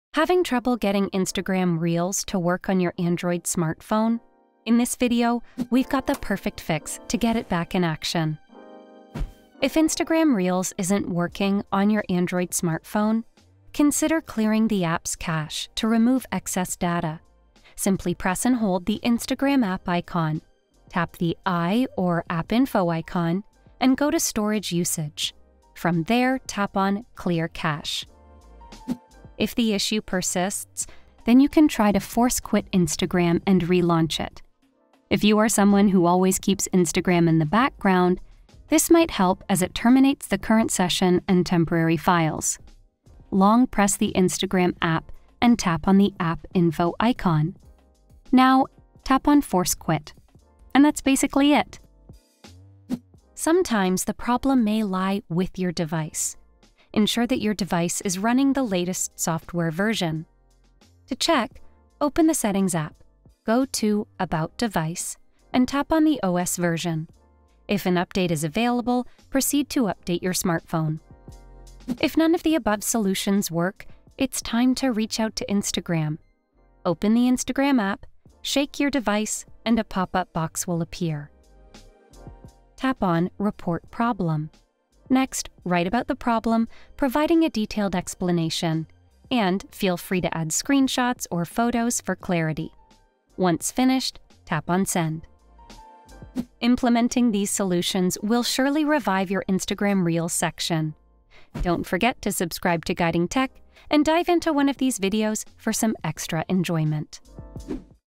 How to Fix Instagram Reels Not Working on Android | Can't Watch Reels on Android?
Ever felt the frustration of not being able to watch Instagram Reels on your Android smartphone? We get it, and we've got your back!

Say goodbye to those worries because, in this video, we're unleashing a variety of solutions to get you back to enjoying Instagram to the fullest.

Dive into the whole video and revive your Instagram experience on Android.

00:00 - Intro
00:09 - Clear the cache
00:29 - Force quit and relaunch the app
00:53 - Check if a software version is available
01:12 - Contact Instagram
01:37 - Outro